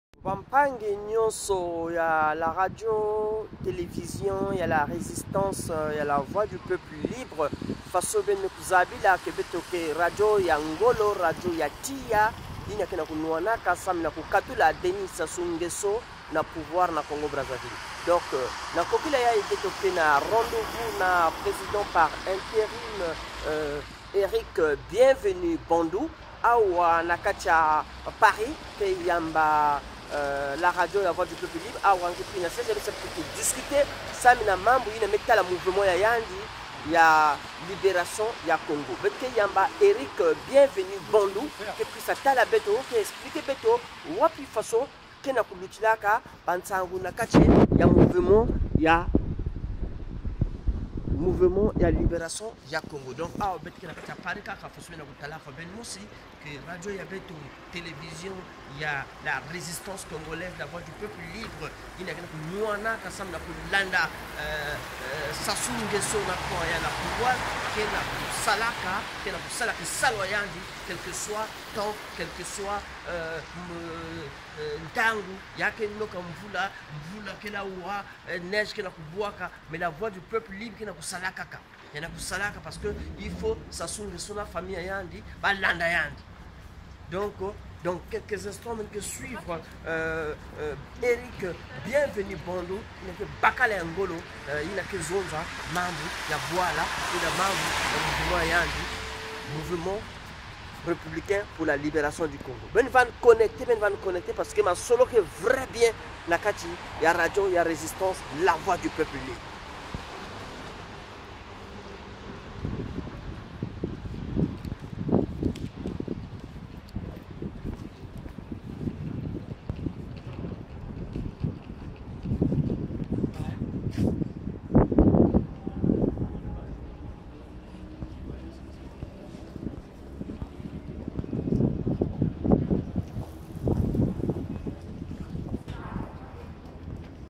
Partie[1] Bienvenue BANDOU nouveau chef de la Résistance
le Nouveau président par intérim du Mouvement congolais pour la Libération du Congo(MRLC). Eric Bienvenue BANDU a été reçu en exclusivité sur la radio Télévision de la Résistance congolaise.